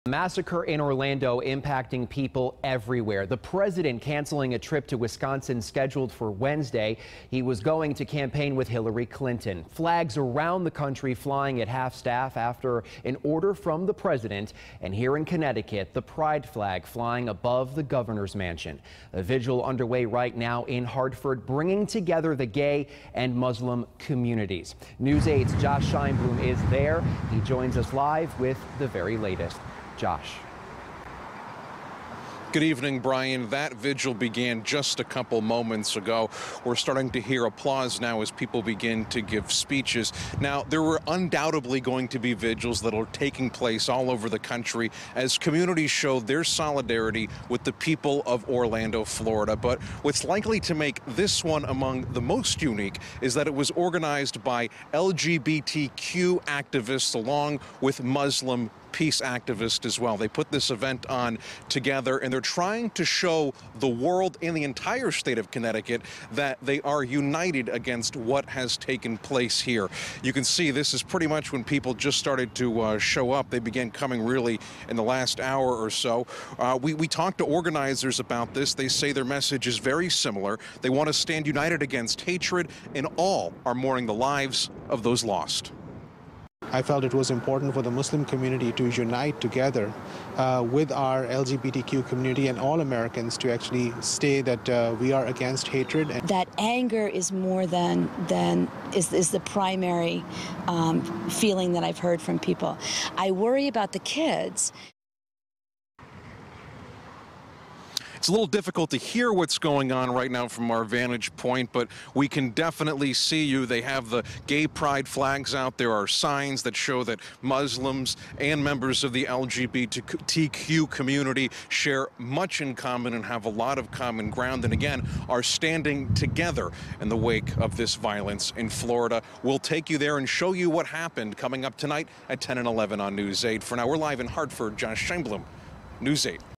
Local LGBTQ & Muslim peace activists hold vigil for Orlando shooting victims
A vigil is underway in Hartford, bringing together the gay and Muslim communities.